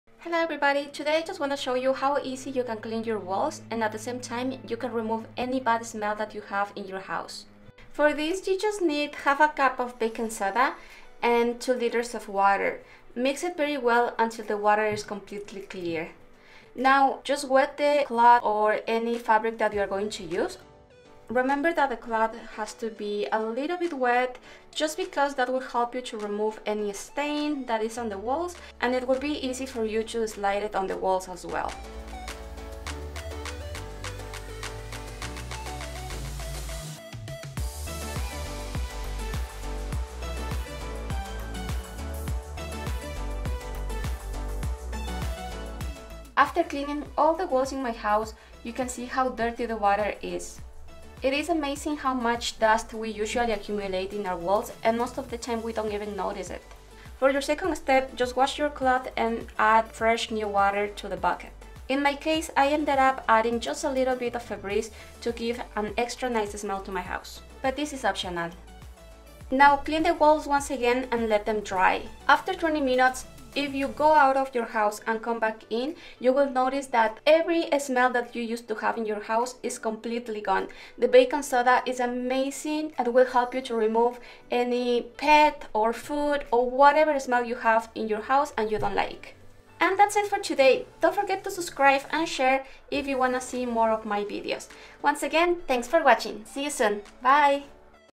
HOW TO CLEAN WALLS / HOW TO REMOVE BAD SMELL IN THE HOUSE
I want to show you how easy is to clean your walls and at the same time you can remove food, pet, or any bad odour from your house or apartment.